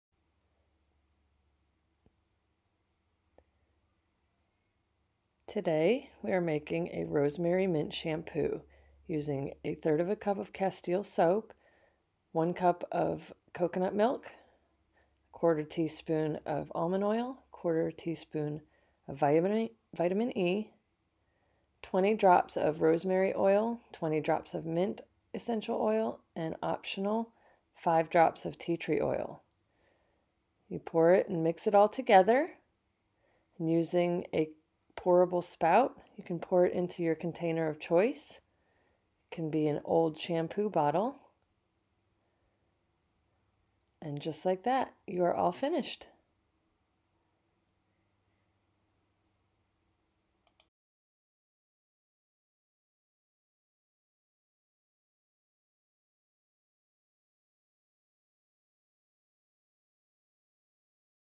Rosemary mint shampoo
Hair health and hair loss is something many of us struggle with.  This Homemade Rosemary Mint Shampoo can help restore the health of your hair.

Rosemary has been used for centuries, and mint for the last 50 years, in the treatment of hair health and excessive hair loss.   There are many store-bought shampoos out on the market today, but you are never too sure exactly what is in them.  Most have the smell of Rosemary and mint, but they have a lot of extras too.  This homemade Rosemary Mint shampoo has just the right amount of rosemary and mint essential oils needed to give you the benefits for healthy hair and scalp without a bunch of added ingredients you do not need.